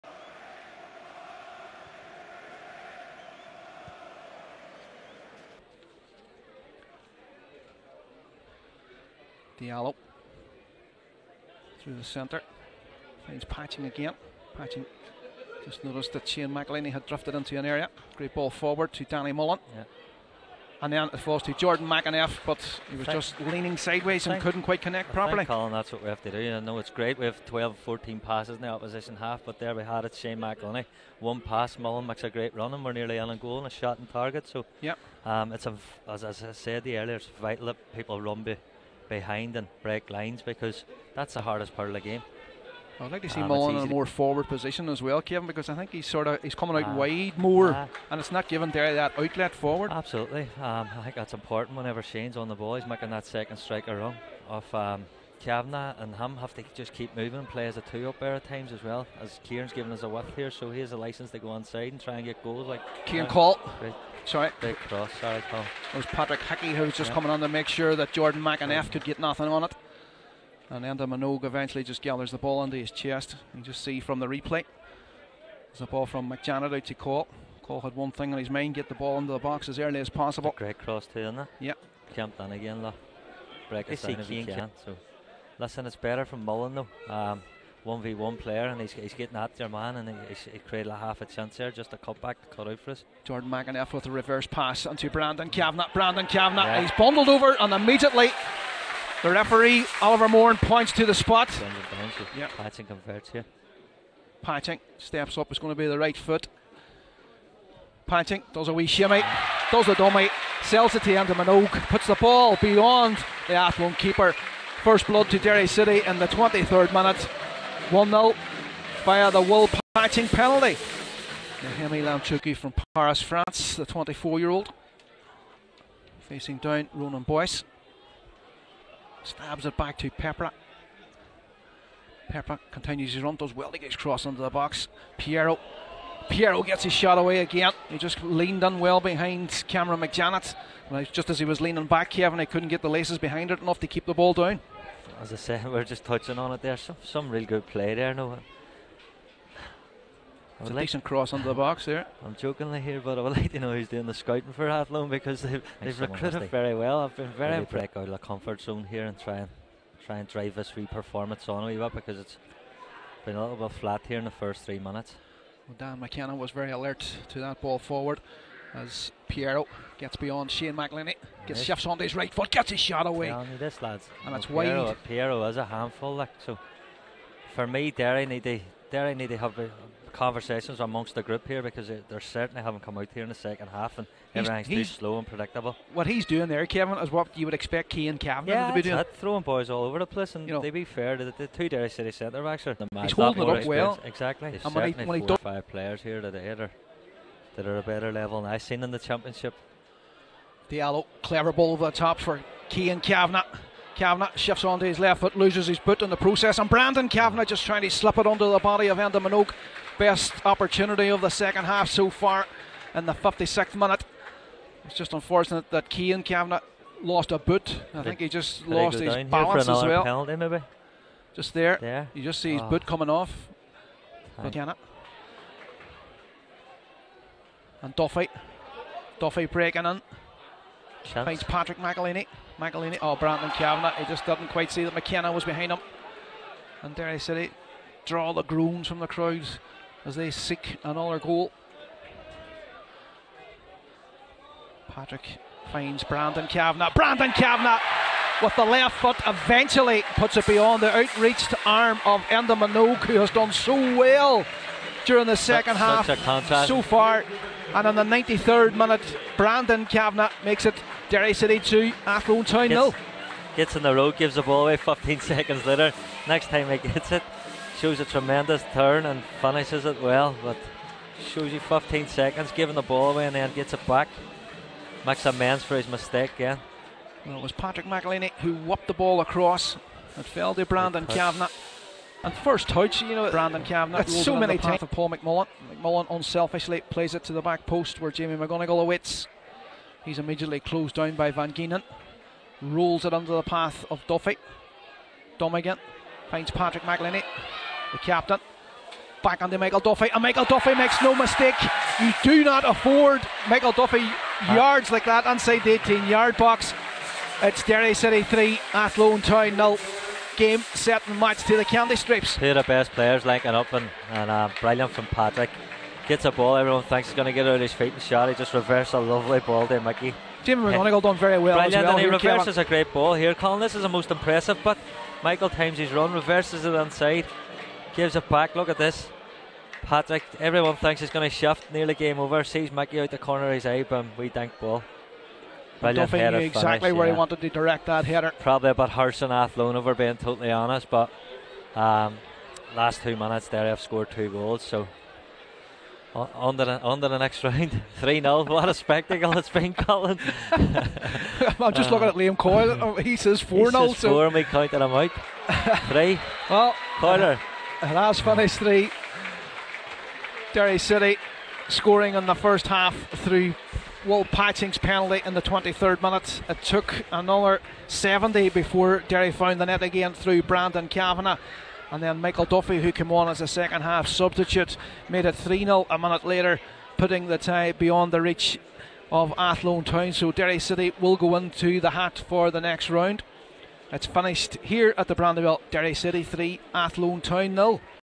Sports Direct Men’s FAI Cup First Round | Derry City 3-0 Athlone Town | Highlights
Defending champions Derry City are through to the second round of the 2023 Sports Direct Men’s FAI Cup after easing past Athlone Town at the Ryan McBride Brandywell.